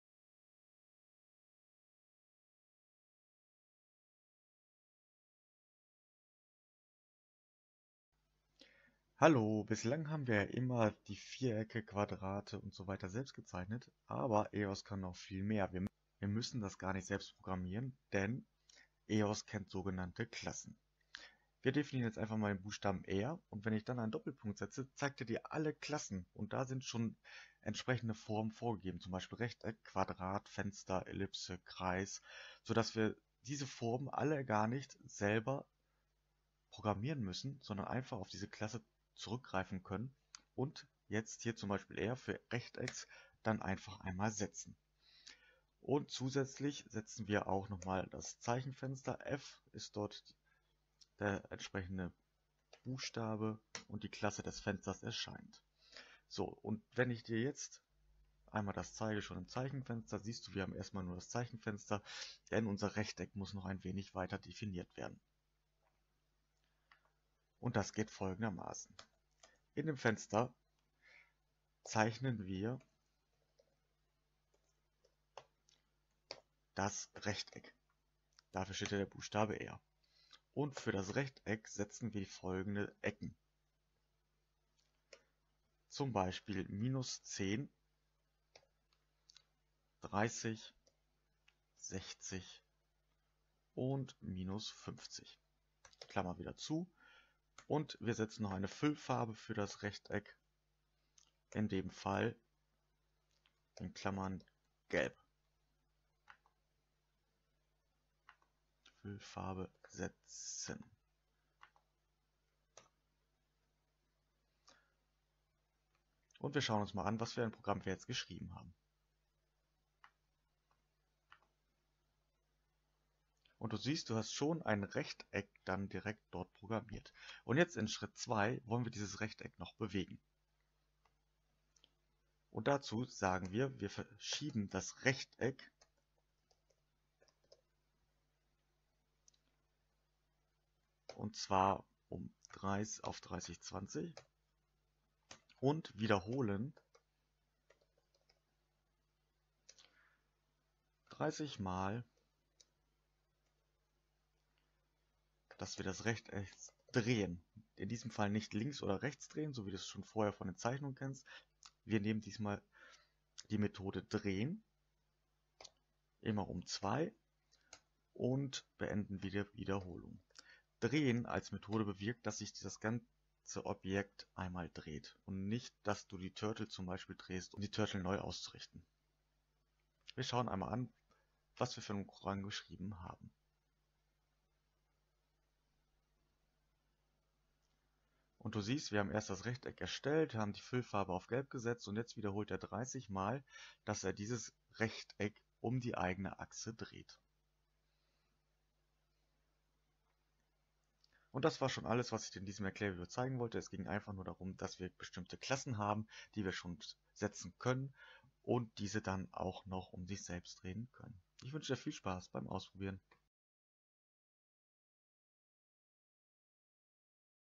05_Klasse Rechteck und Objekt drehen
Hier wird dir gezeigt, wie du auf die Klasse "Rechteck bei EOS zurückgreifst, damit du die Rechtecke nicht selbst programmieren musst. Anschließend wird das Objekt "Rechteck" um die eigene Achse gedreht.